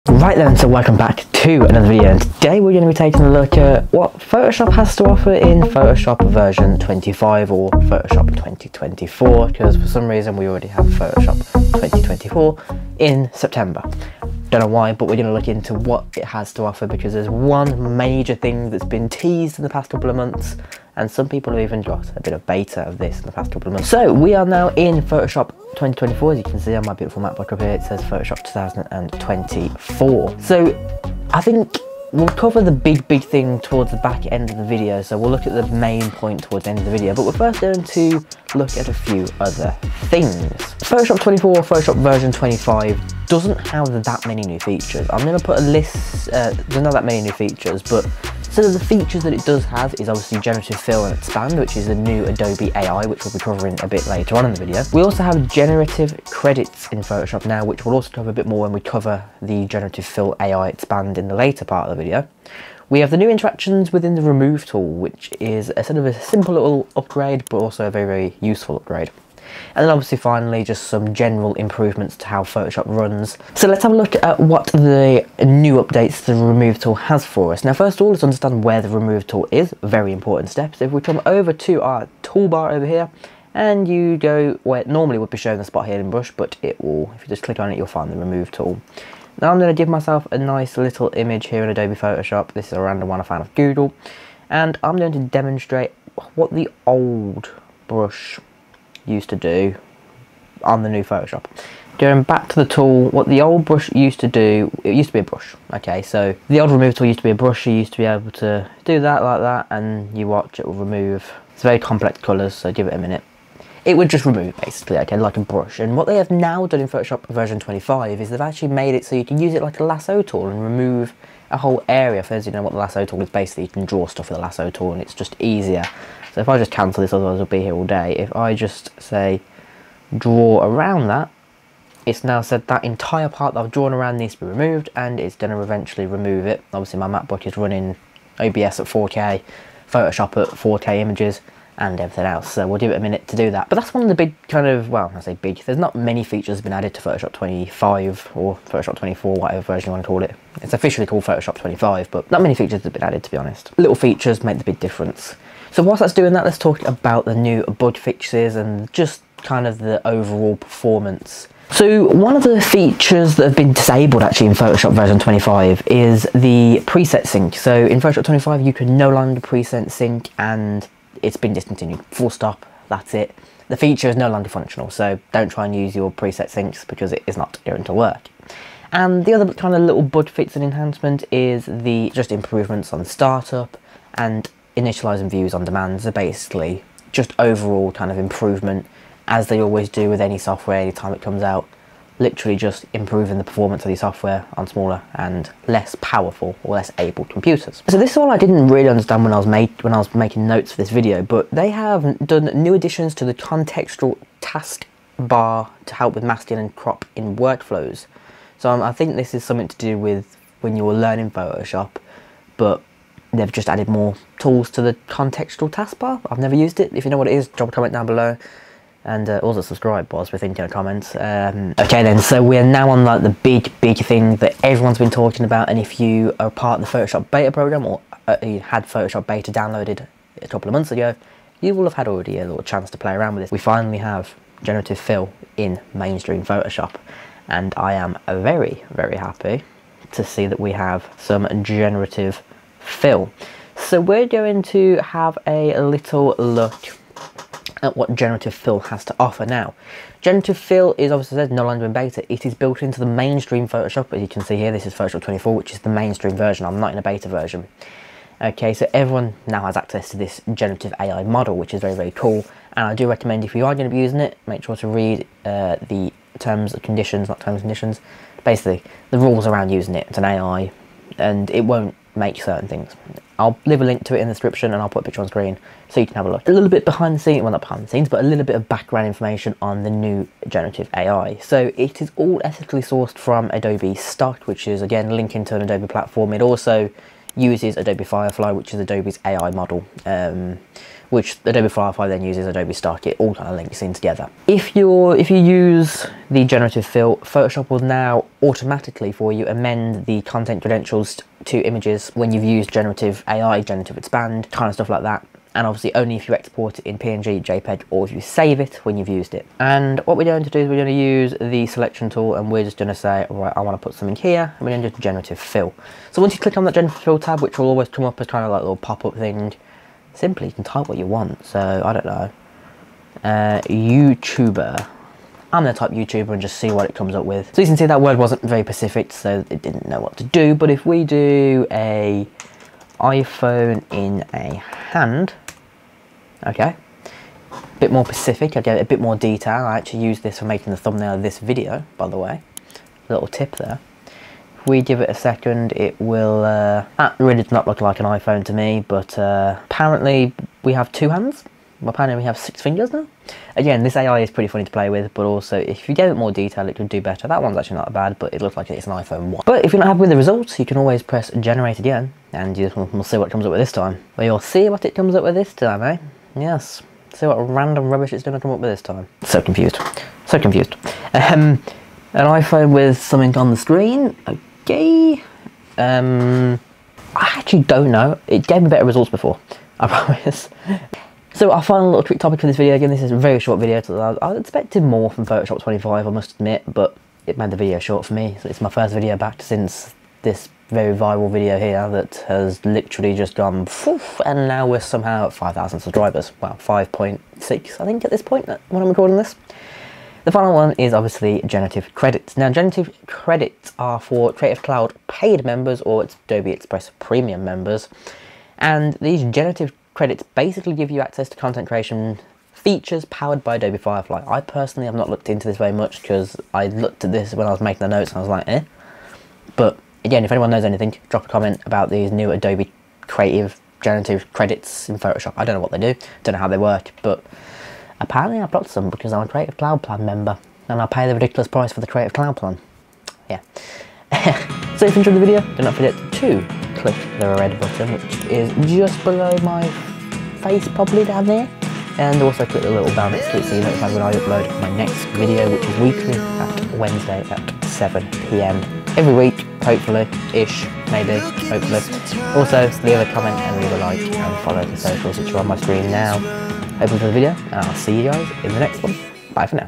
Adobes AI is VERY Werid!
Photoshop 2024 IN SEPTEMBER!!! What does it have to offer? Stick around till the end of the video to see what random things Adobes AI comes up with!